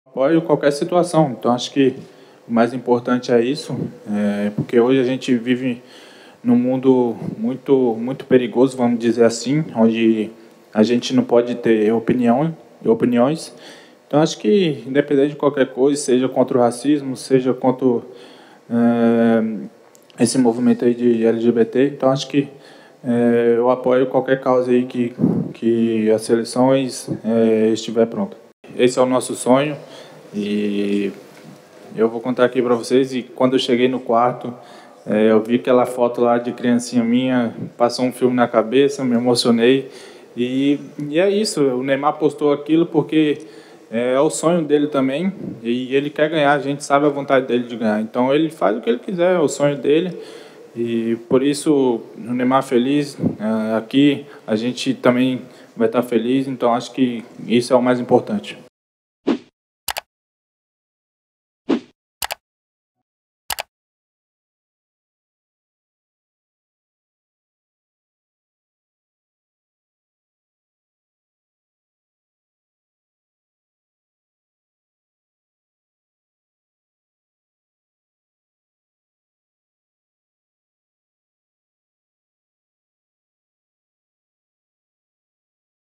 'We can't have opinions! It's a very dangerous world' | Richarlison on his stance
Brazil Richarlison said that he is supportive of the anti-racism and LGBTQ movement causes during a news conference at their camp in Doha.

He also defended teammate Neymar after the German newspaper Bild critisized him for publishing a photo adding a sixth star to the Brazil crest, symbolizing the five World Cups that they have won.

FIFA came under pressure Wednesday from several European soccer federations who want their captains to wear an armband with a rainbow heart design during World Cup games in Qatar to campaign against discrimination.

France and Germany, the last two World Cup champions, were among eight of the 13 European soccer teams going to Qatar who joined the "OneLove" campaign, which started in the Netherlands.

Just hours before the first players with the armbands in support of the "One Love" campaign were to take the field, FIFA warned they would immediately be shown yellow cards — two of which lead to a player's expulsion from that game and also the next.

That changed the calculus for the seven European teams, which may have expected merely to be fined.

The displays are a violation of FIFA rules.